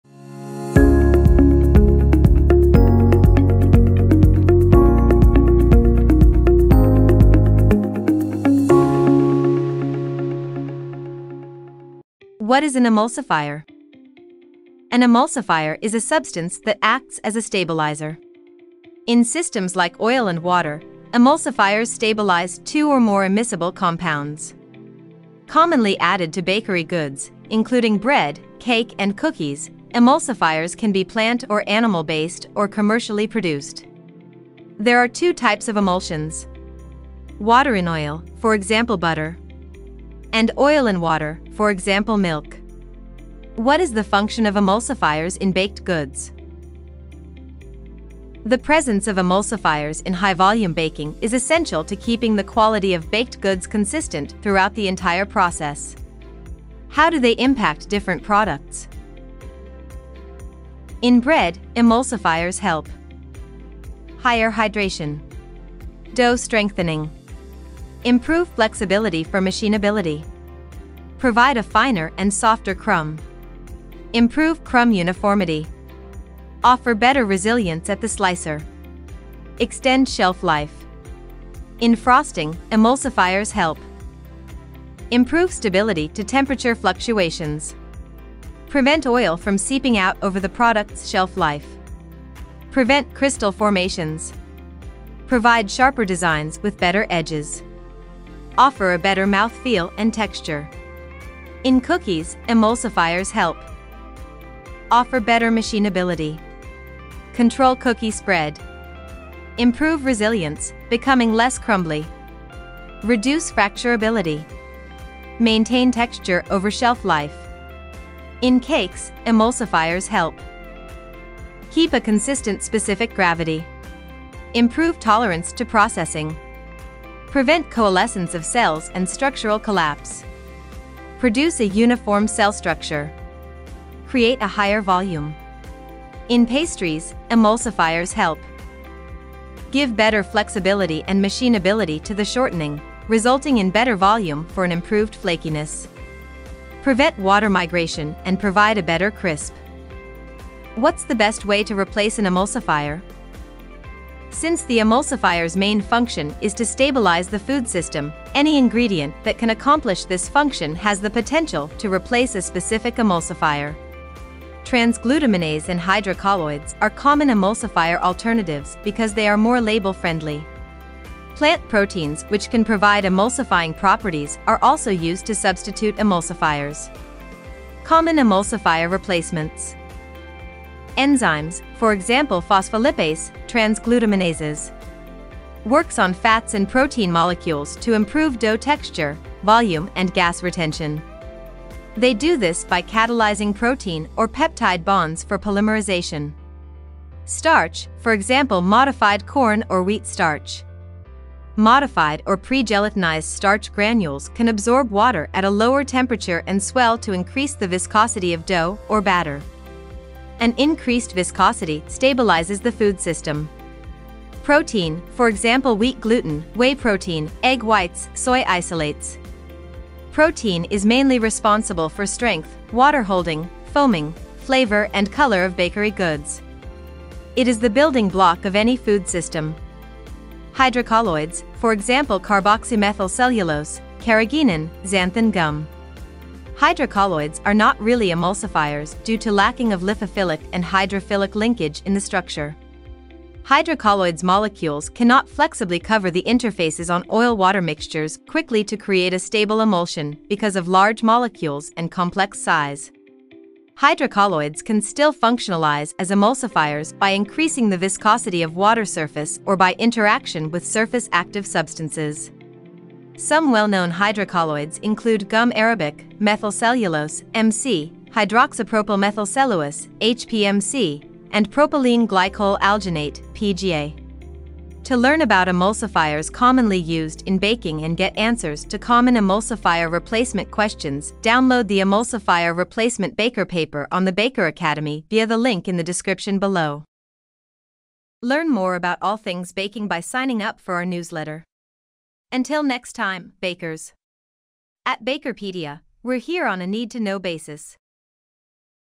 What is an Emulsifier? | Knead to Know Basis | BAKERpedia
An emulsifier has a water-loving end and a fat-loving end. It is used in water-oil emulsions to prevent their immiscibility and to maintain these systems’ stability. In the baking industry, emulsifiers are used frequently, but surprisingly, not everyone knows their functionality. Emulsifiers are mythologized, expected to strengthen gluten network and improve structure. Find out what you need to know about emulsifiers and how to replace them in this video!

Curious to learn about more ingredient replacement?